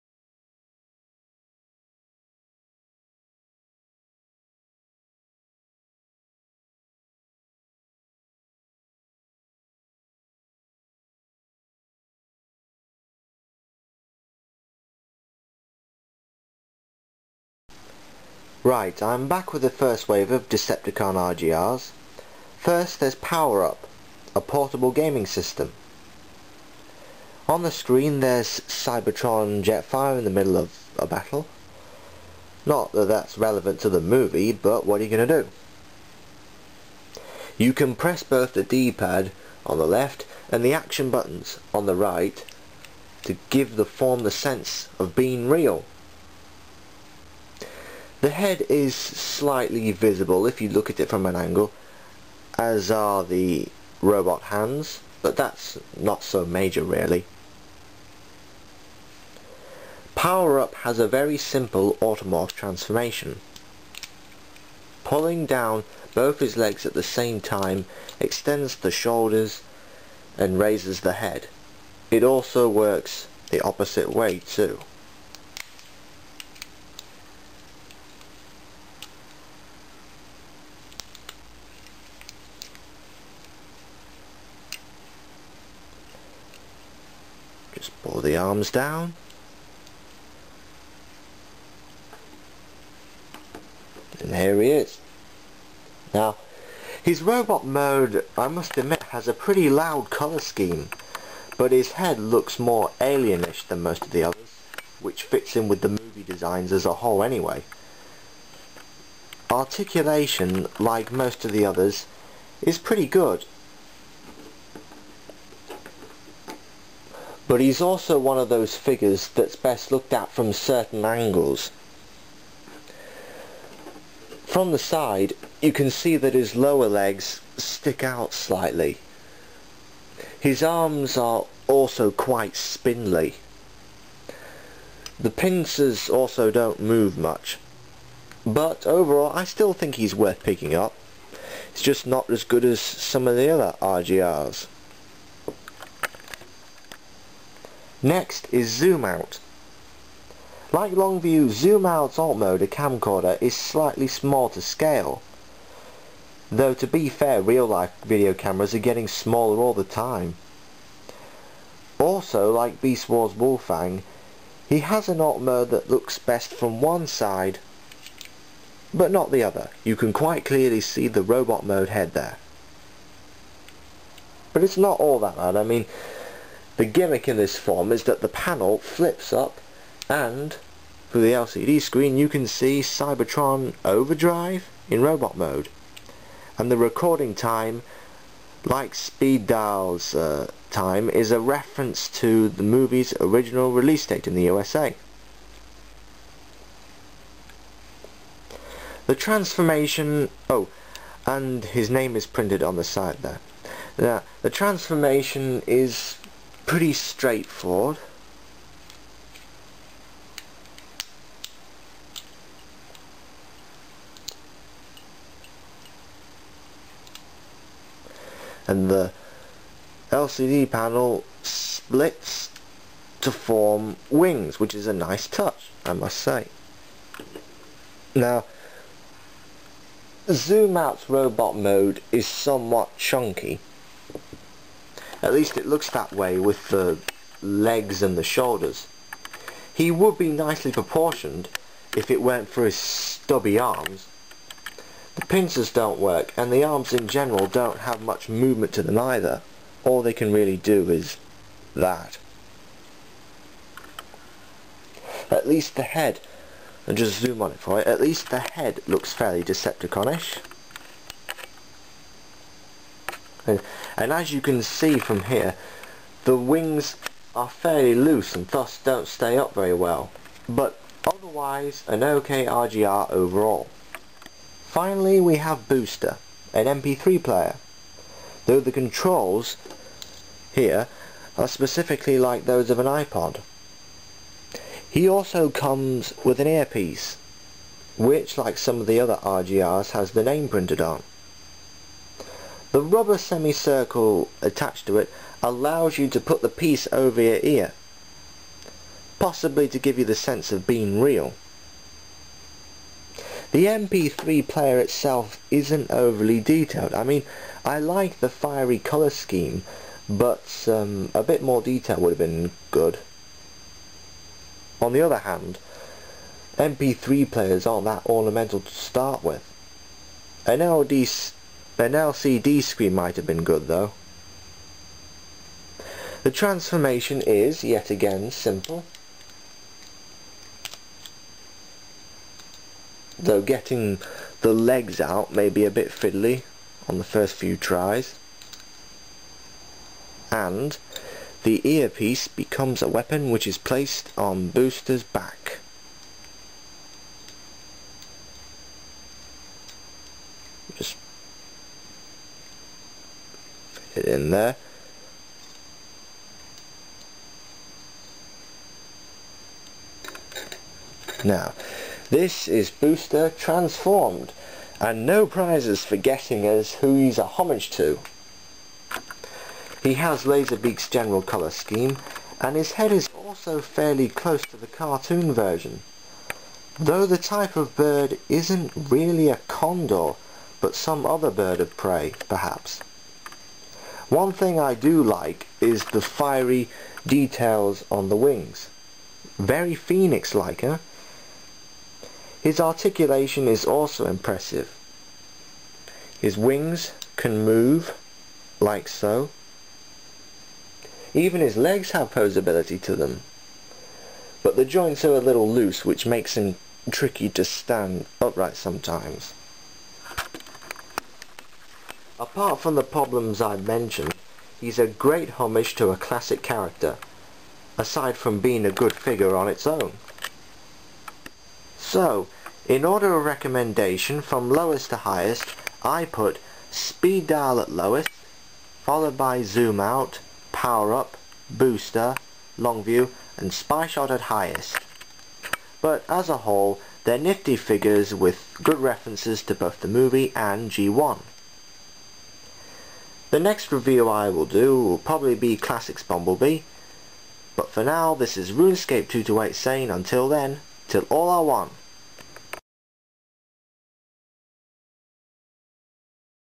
Transformers - "Movie [2007]" Real Gear Robots [Decepticon, Wave #1 & 2] Review
Originally intended for the "Cybertron" range, the Real Gear Robots are a solid batch of figures; some stronger than others.

This video covers:
* Power Up VT6
* Zoom Out 25X
* Booster X10